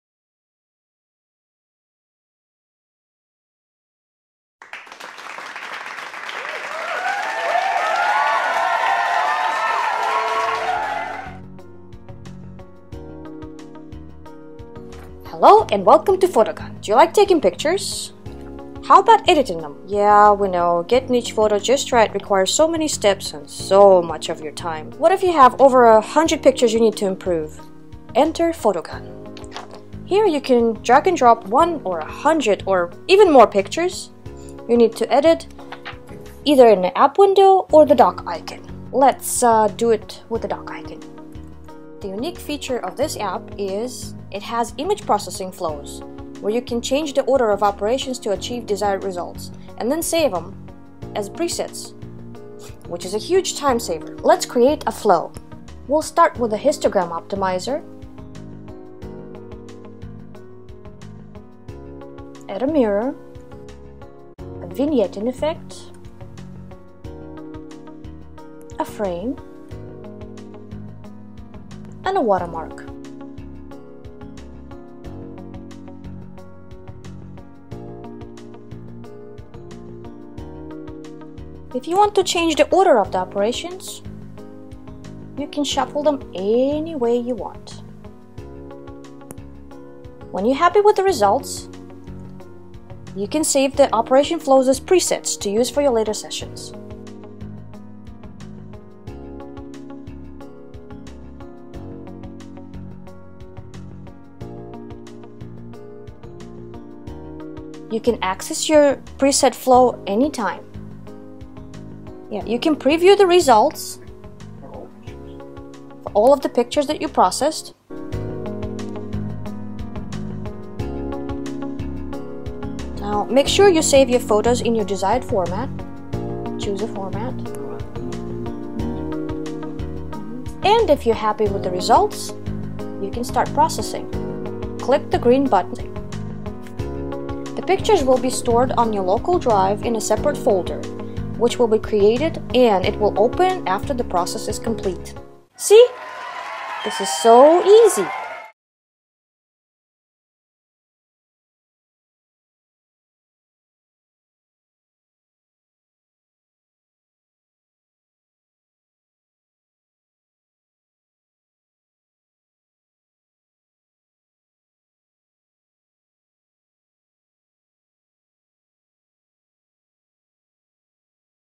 Powerful Tool for Photo Processing: Watermark your Photos, Convert your Photos and more.
Do you like taking pictures?  How about editing them?  Yeah, we know, getting each photo just right requires so many steps, so much of your time… What if you had over a hundred pictures you needed to improve?  Enter PhotoGun. Simply drag and drop one, or a hundred, or thousands of photos you need to edit to the drop window and select one or multiple operations you want to perform on them. Click the green button and here they are, your processed photo files stored in the separate output folder.

Here’s what you can do:
- Resize to make the photos more suitable for email, blog or any other web, personal or marketing use.
- Adjust Brightness, Contrast and Saturation to enhance the light and color balance.
- Add a professional touch to your photos and apply “Vignetting”, a creative effect, to draw attention to the center of the frame.
- Crop to cut away unneeded image parts.
- Rotate to change the image orientations.
- Use Mirror to flip the image vertically and horizontally.
- Blur to hide some level of detail in your photos.
- Add Borders for a perfect look, using different frame thickness and colors.
- Swap the RGB-channels.
- Apply the Histogram optimization to automatically improve photos taken under similar conditions.
- Binarize images using the threshold value, and produce monochromatic results.
- Use Grayscale to convert color images into black/gray/white representation.
- Invert photos.
- Convert the file format to/from JPEG, PNG, TIFF, BMP, XPM, PPM, including the full range quality compression of JPEG ranging from 1% to 100%.
- Give new titles to your photos and use flexible choice which allows you to add odd, even and all-number titles at the end of file names.
- Change operations order to achieve desired results.
- Save your operation flows as presets that you’ll use over and over again.

The App has a Real Time photo editing preview with retina-ready graphics.

The Real Time photo editing preview will help you do all this:
- Preview all images at each stage of applied operations by clicking the eye symbol on the flow thumbnails.
- Preview resulting photos in a small window, as well as in the embedded previewer using the original image size,
- Preview resulting photos during the creation process in the batch dialog.

We hope you will have as much fun using “PhotoGun” as we had creating it for you! Click to buy and enjoy your perfect pictures!